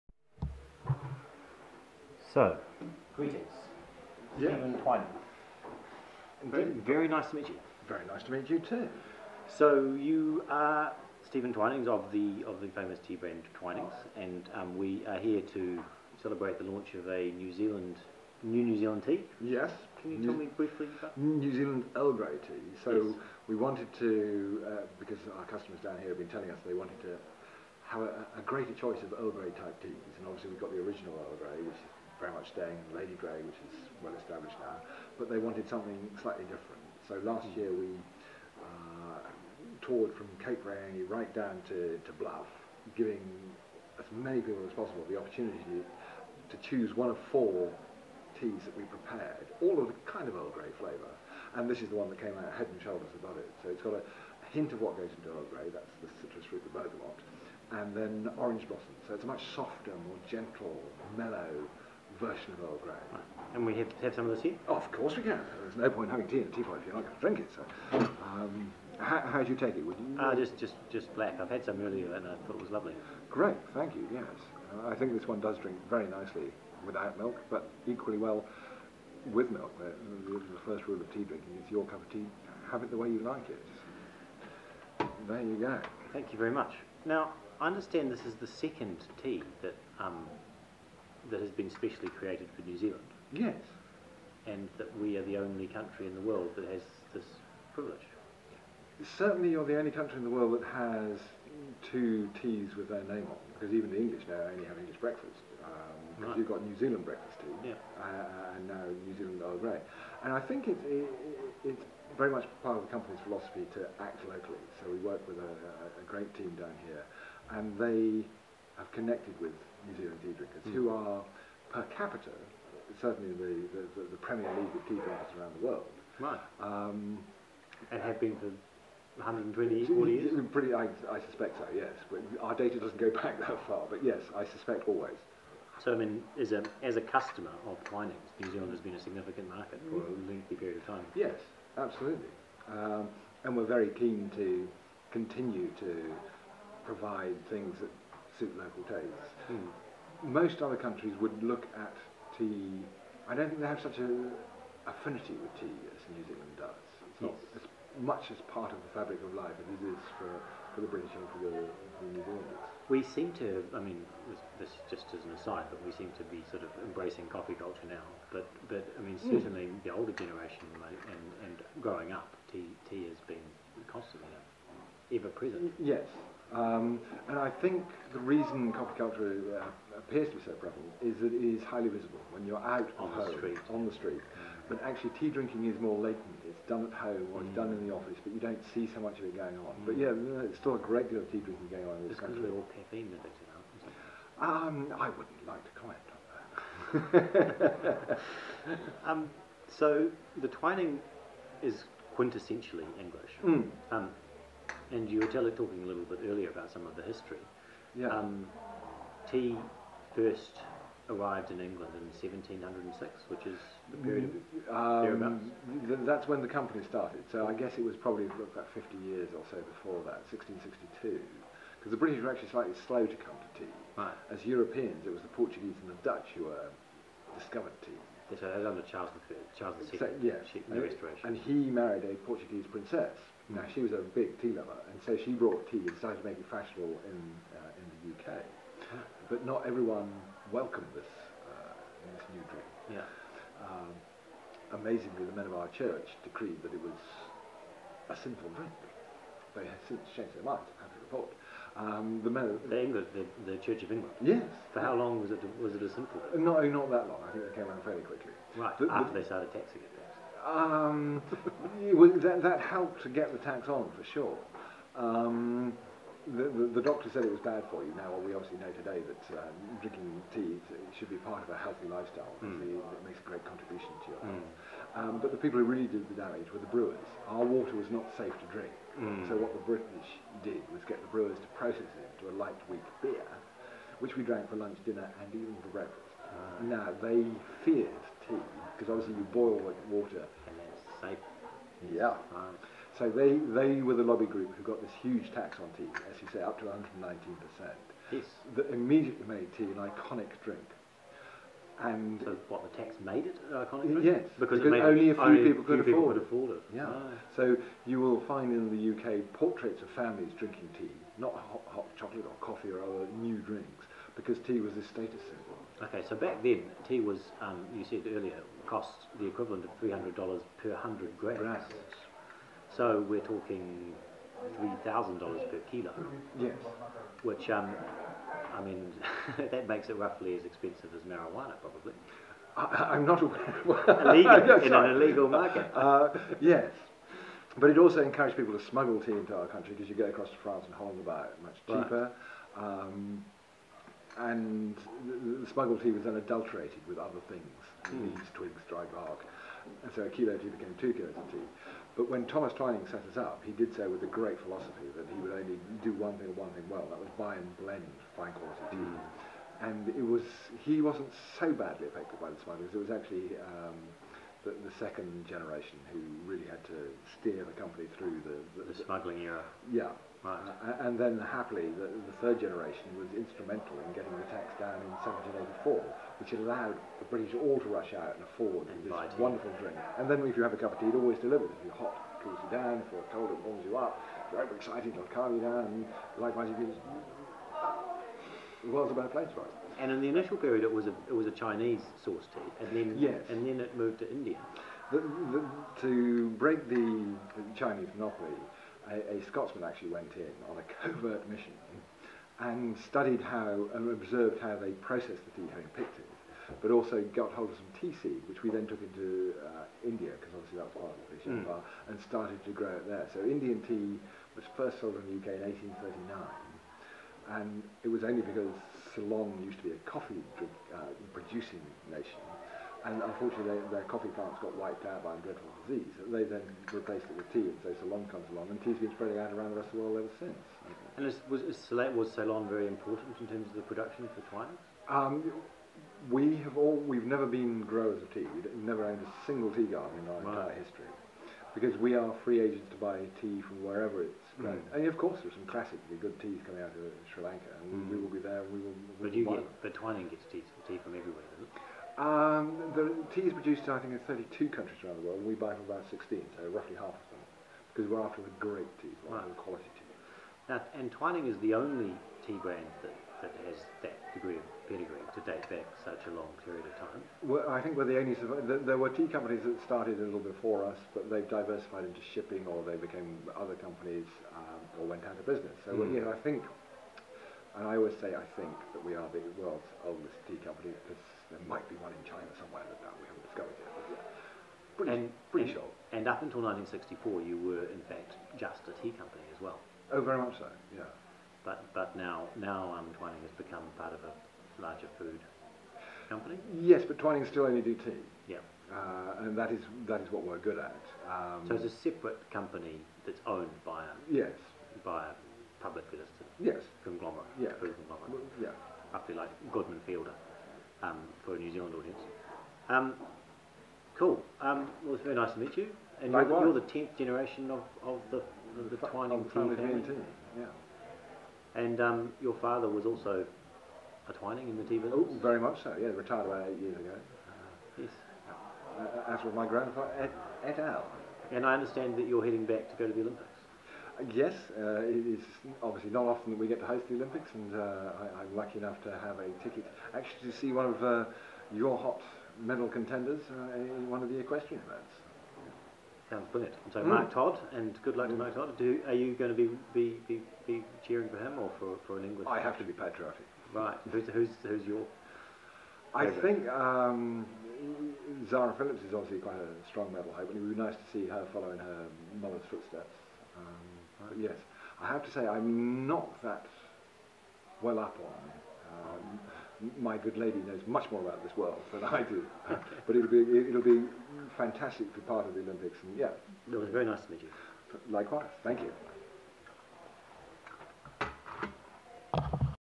Stephen Twinning Interview With Scoop's Alastair Thompson On Launch Of NZ Earl Grey Tea
Scoop's Alastair Thompson interviews 10th generation tea baron Steven Twinning at Homewood, the home of the British High Commissioner in New Zealand on the ocasion of the launch of New Zealand Earl Grey Tea in New Zealand.